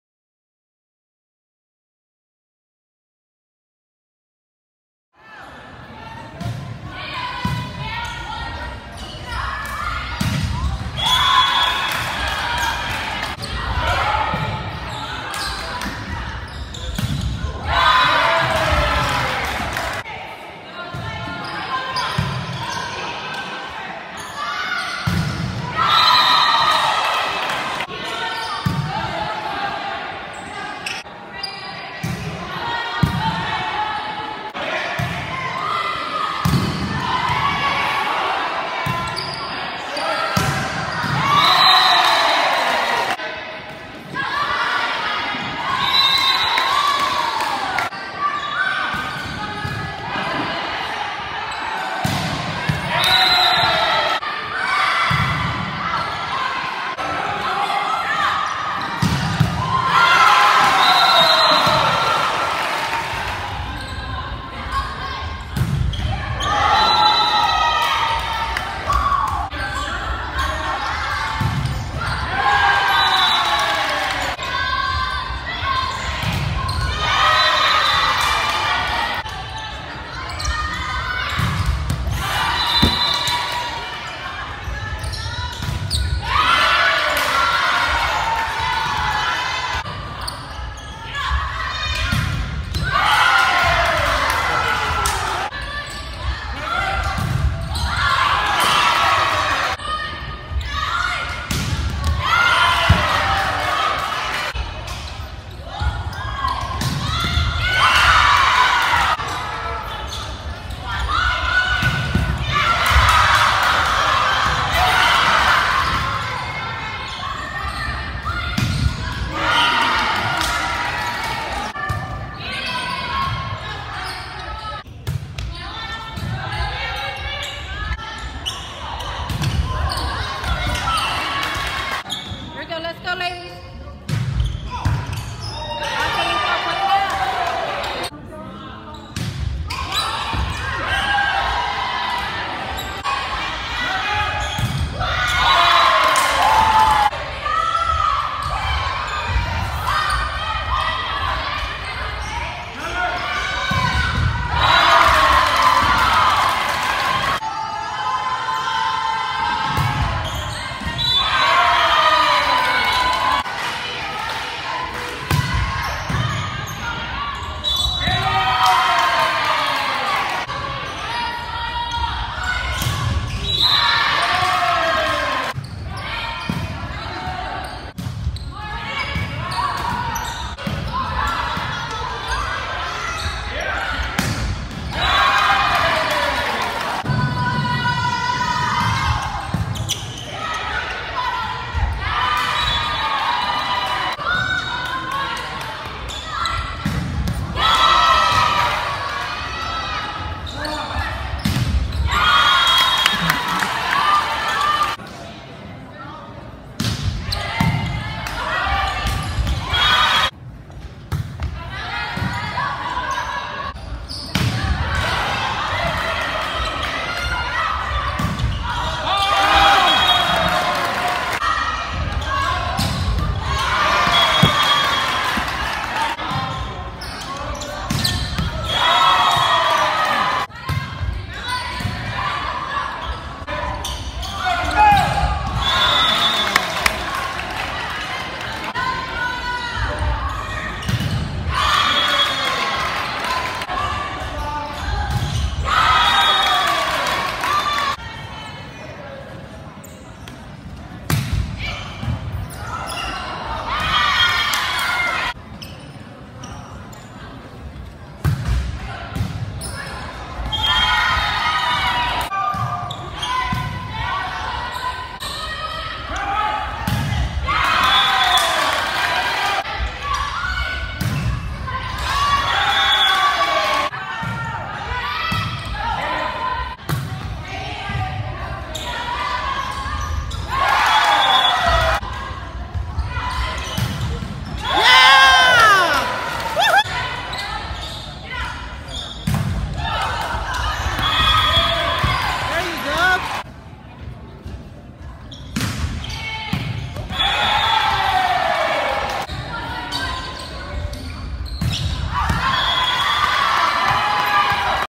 Mele Corral-Blagojevich 2019 Ann Kang Tournament Highlights
Freshman Outside Hitter , Mele Corral-Blagojevich, makes her Varsity High School Debut as the Redondo Union Sea Hawks win the 2019 Ann Kang Tournament in Honolulu, Hi and play against Powerhouses like Los Alamitos, Santa Margarita, Iolani, and Byron Nelson